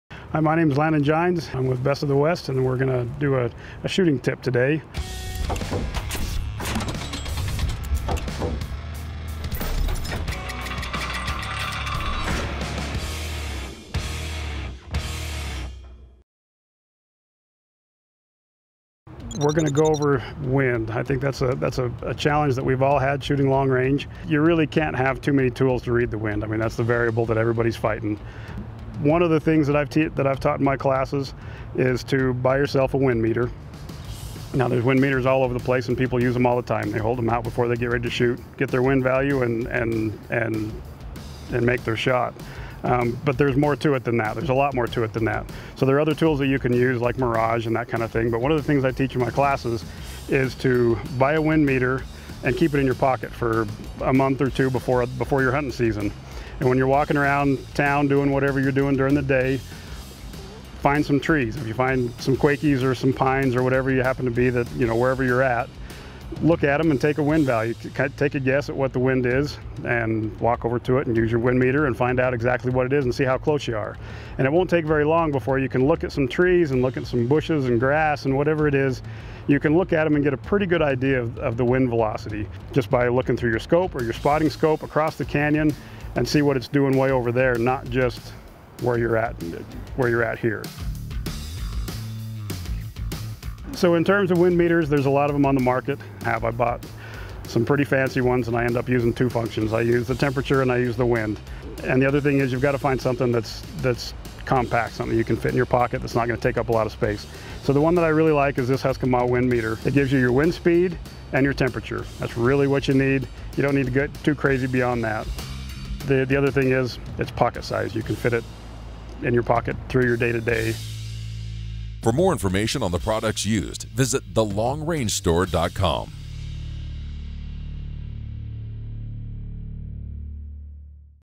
Wind Reading Tip
Landon Gines gives us some great wind reading tips.